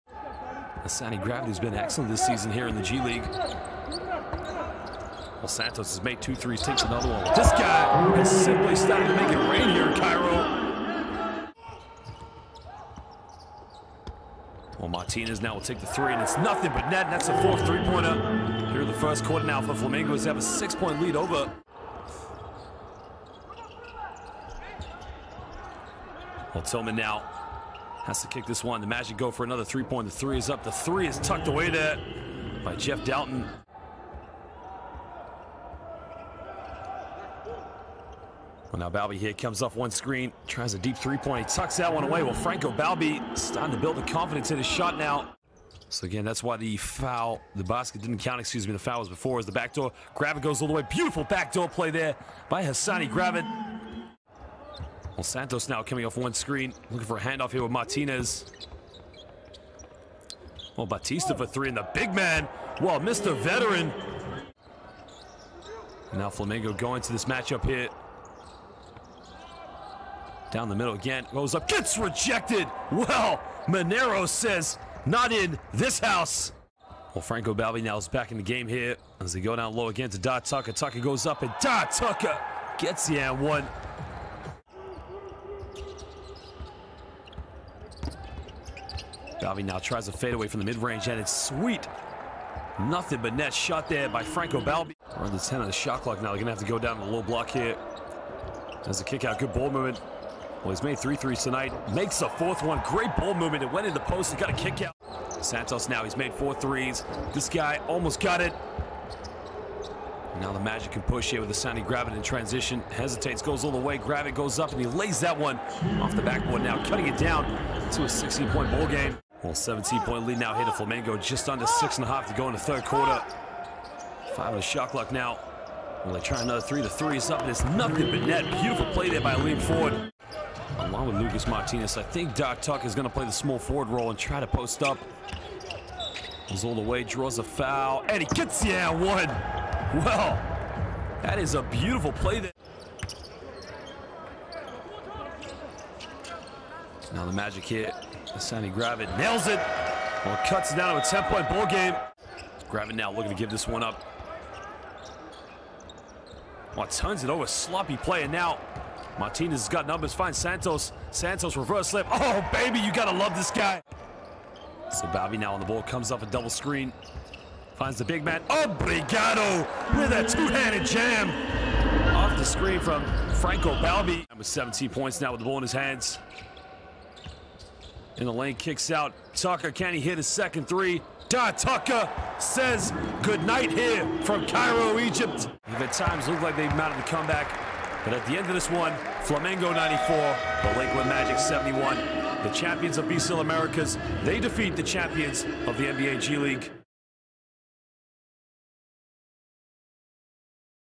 Flamengo - Lakeland Magic | Highlights - Semi-Final | FIBA Intercontinental Cup 2022
Enjoy the highlights of the game between Flamengo and Lakeland Magic from the Semi-Finals of the FIBA Intercontinental Cup 2022!